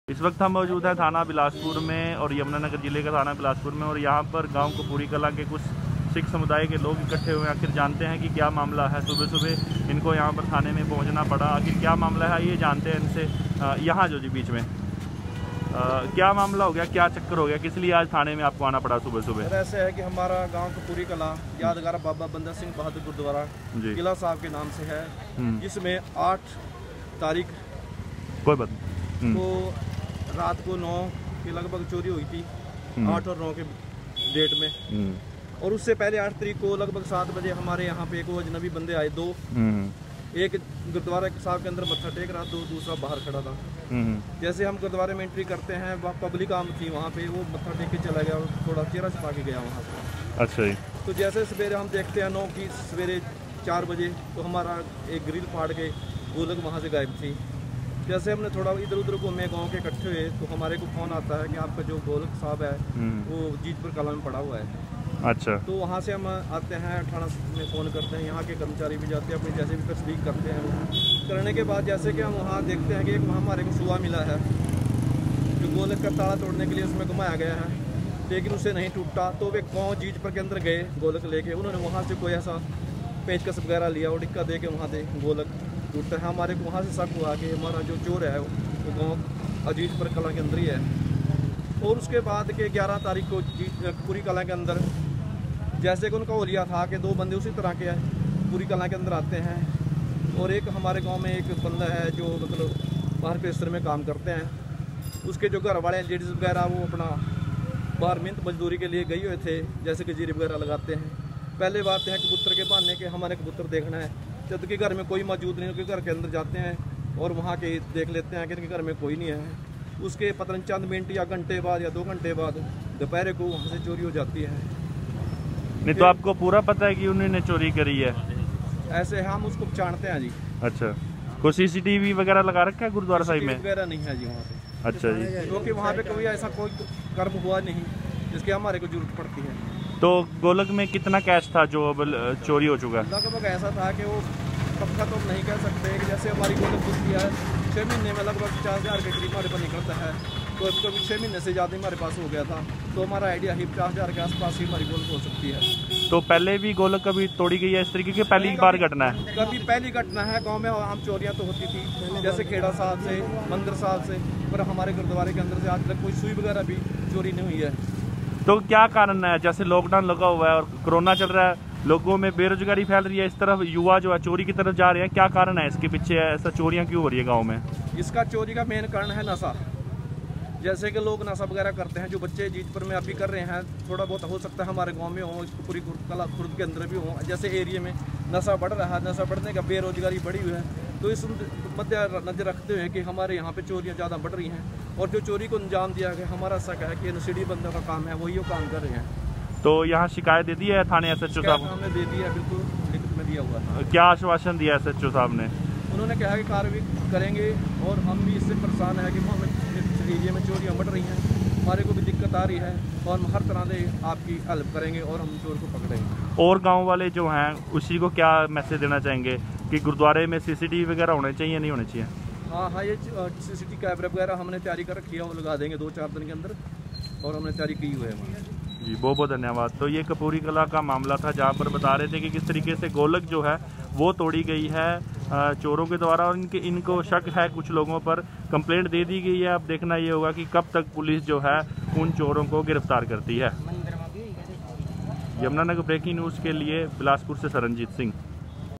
Gurudwara chori in kapoori kalan Yamunanagar
YouTube Top 10 Video Links

सबसे सस्ती सब्जी की मार्केट जहा ₹2 किलो टमाटर Live: देखे: यमुनानगर के खंड बिलासपुर के गांव कपूरी कला में अज्ञात चोरों ने गुरुद्वारा साहिब की गोलक को निशाना बनाया। गोलक से नकदी चोरी करके फरार। गुरुद्वारा कमेटी के सदस्य पुलिस स्टेशन पहुंचे शिकायत दर्ज करवाने।

FACEBOOK-Group ko Join kare